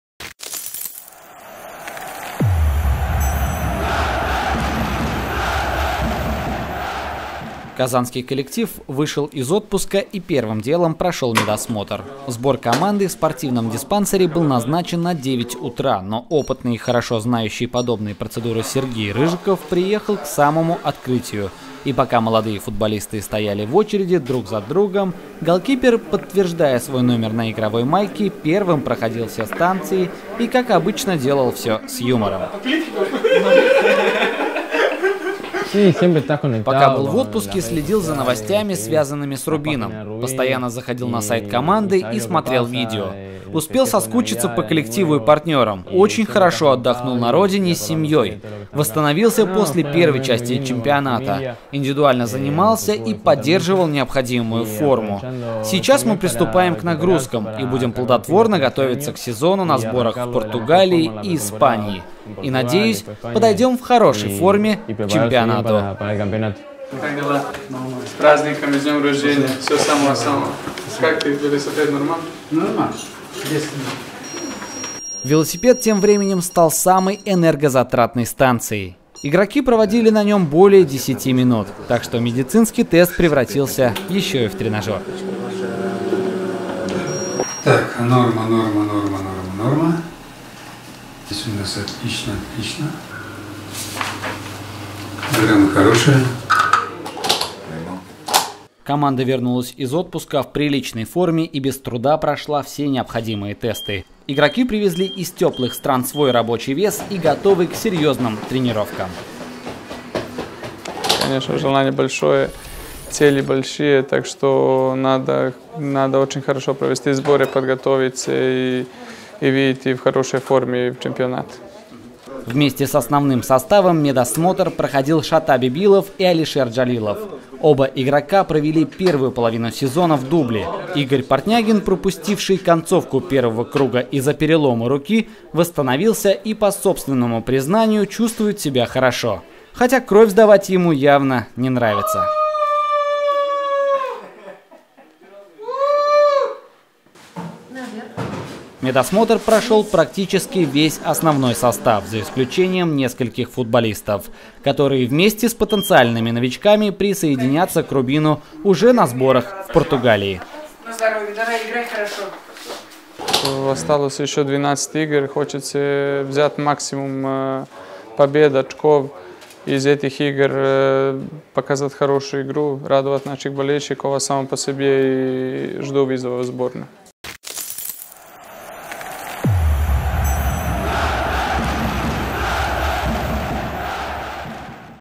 Портнягин - первая кровь, или как «Рубин» прошел медосмотр
Казанский коллектив вышел из отпуска и  первым делом прошел медицинское обследование